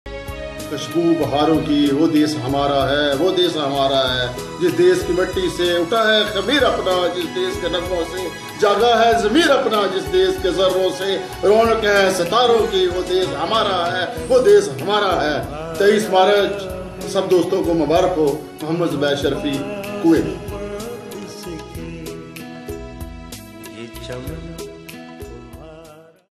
Youm e pakistan zubair Sharfi 2016
محمد زبیر شرفی صدر پاکستان مسلم لیگ لیبرونگ اووزسیزکویت کی جانب سے 23مارچ یوم پاکستان کے پر مسرت موقع تمام اہل وطن کو مبارک باد